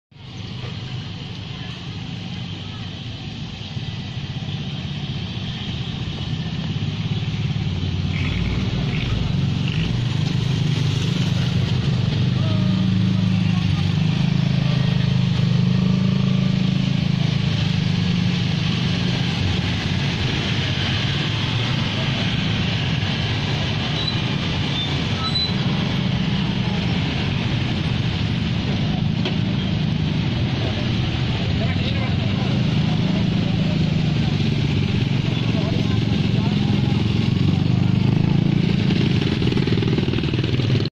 Unik, DPD Partai Perindo Solo Datangi KPUD Surakarta Diantar Gojek
Pendaftaran Partai Perindo ke KPU secara serentak dilakukan pada Senin (9/10). Ada sebuah hal unik yang terjadi saat DPD Partai Perindo Solo mendatangi kantor KPUD Surakarta. Rombongan pengurus dan kader Partai Perindo Solo diantar dengan becak hias dan ojek online hingga ke depan KPUD Surakarta untuk menyerahkan berkas pendaftaran peserta Pemilu 2019.